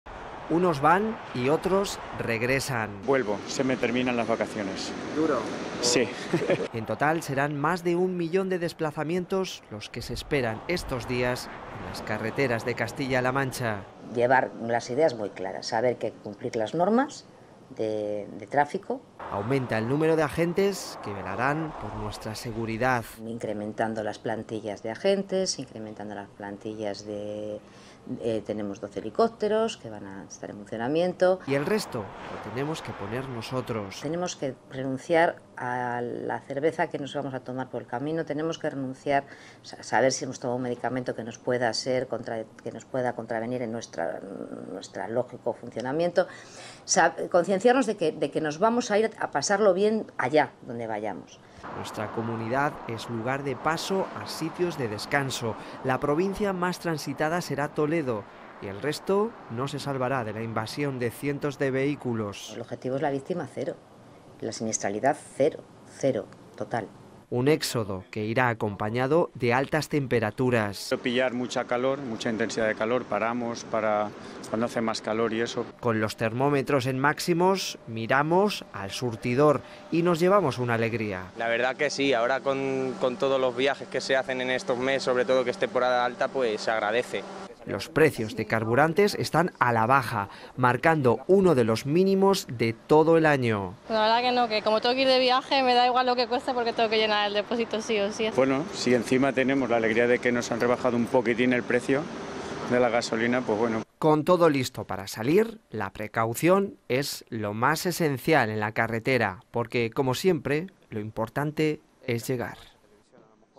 COMIENZA LA OPERACIÓN SALIDA DE AGOSTO, LA MÁS IMPORTANTE DEL AÑO
Es la operación de tráfico más importante del año. Alrededor de un millón de vehículos van a circular estos días por Castilla-La Mancha. Precaución y sobre todo, mucho cuidado en los desplazamientos cortos. Para aquellos que antes de salir tienen que llenar el depósito de gasolina se llevarán una grata sorpresa: el litro de carburante es uno de los más bajos de lo que llevamos de año.